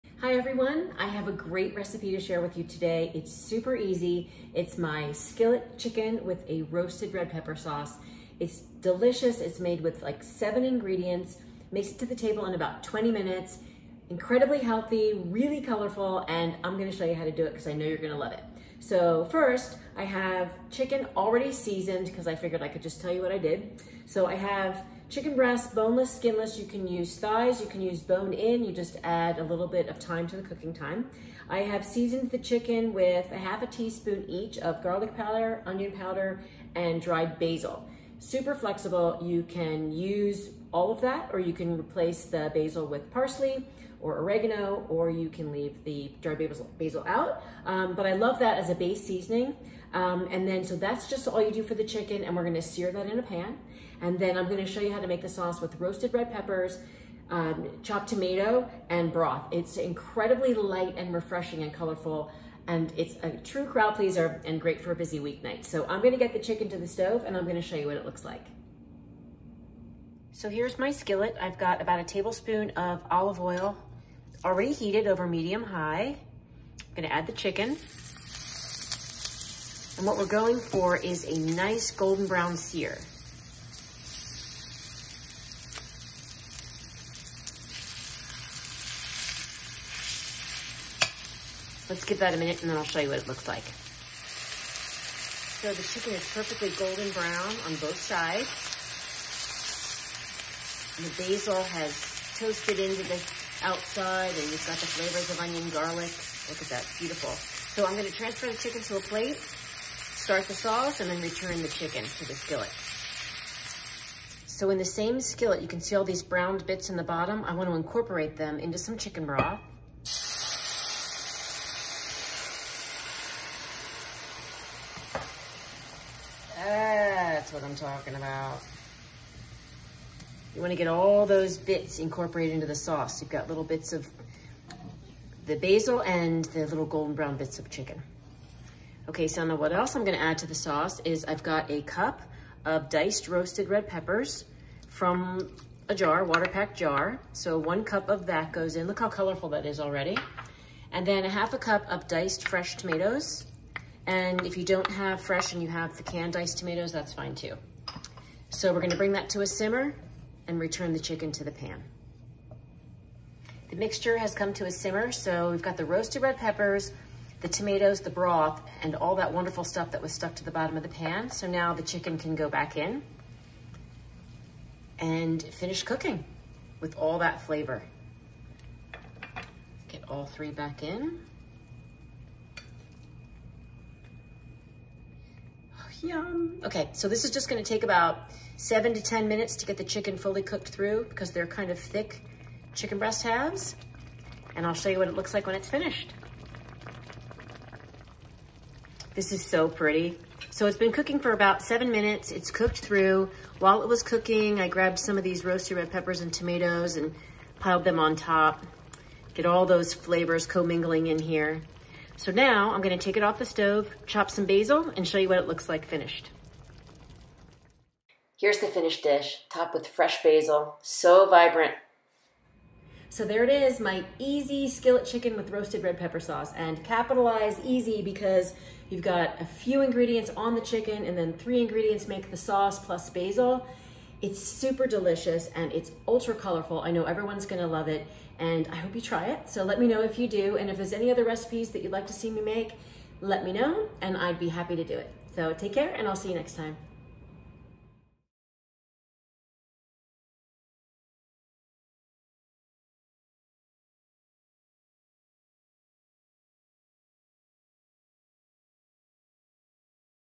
Skillet Chicken with Easy Roasted Red Pepper Sauce
A truly amazing chicken dinner! Pan-seared, golden brown chicken is infused with garlic, onion and basil and then simmered in a robust, smoky, roasted red pepper/tomato sauce. Healthy, vibrant, gluten-free, and ready in less than 20 minutes!